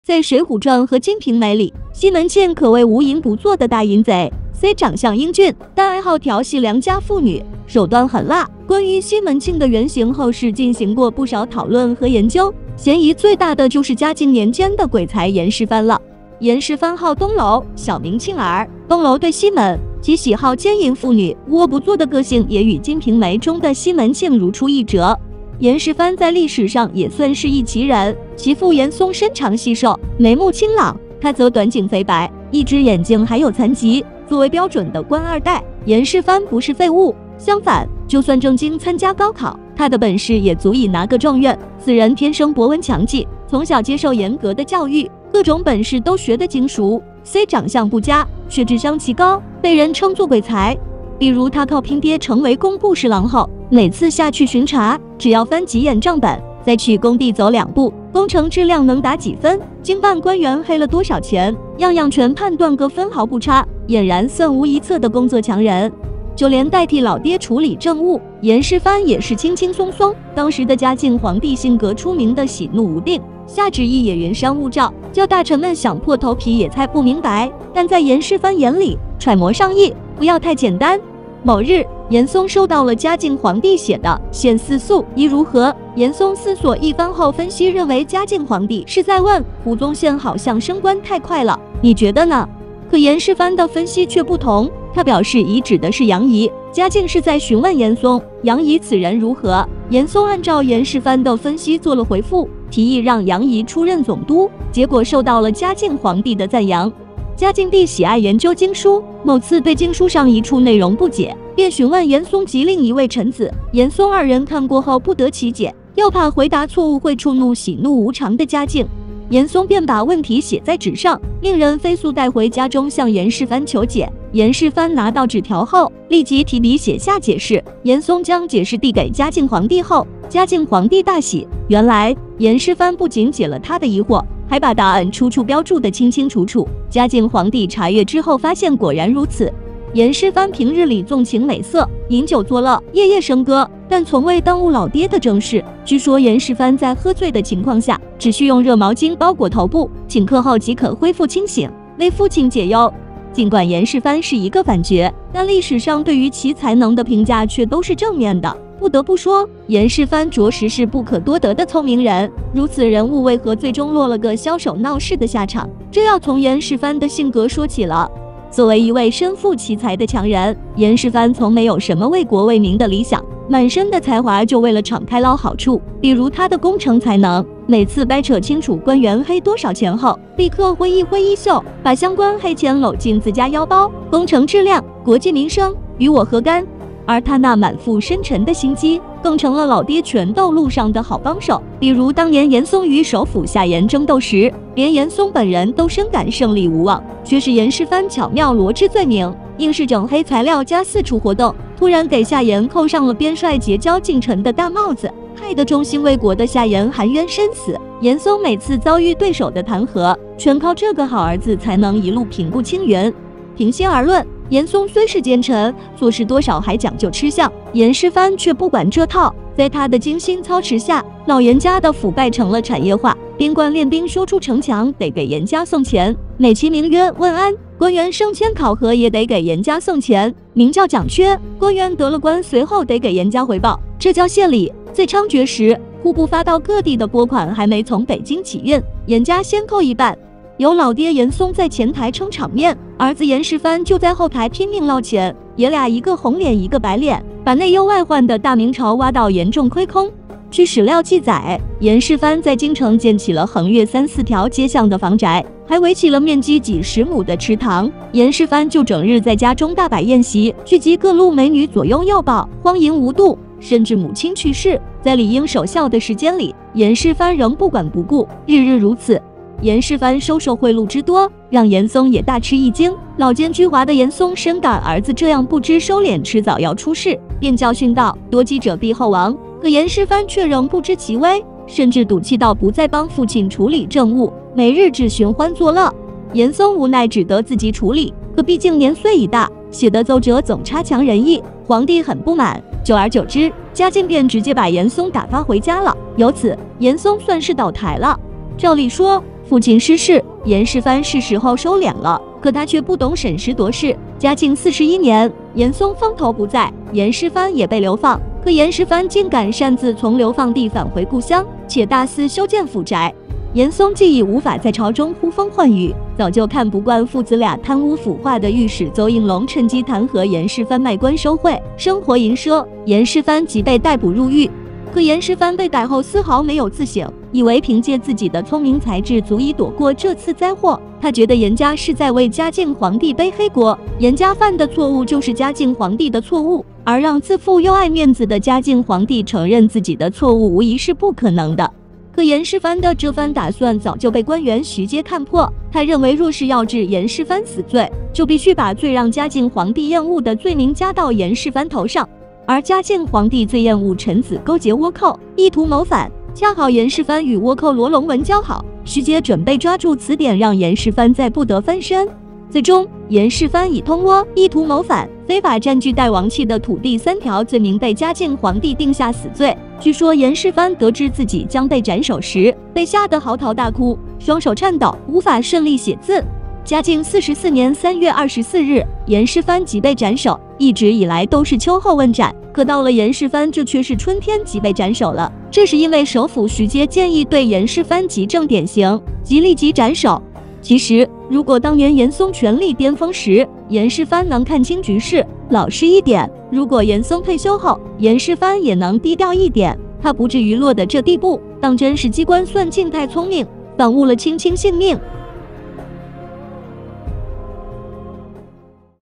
《金瓶梅》中西门庆的原型竟然是他？曾被称为嘉靖王朝第一鬼才，从权倾朝野到被斩首于闹市，他只用了15年【猫眼观历史】
严世蕃被称为嘉靖朝中的第一鬼才，即使放眼整个明朝，他也是个不可多得的绝顶聪明的天才人物。他的天才不仅体现在过目不忘上，更是玩弄权术的一把好手。他十几年来在朝堂党争中立于不败之地，扳倒了无数政敌。如此精于算计的他为何最终被别人算计，丢掉了性命？欢迎收看本期视频。

00:00 为何说西门庆的原型是他
00:50 严世蕃的天才之处
02:46 严世蕃的自我毁灭之路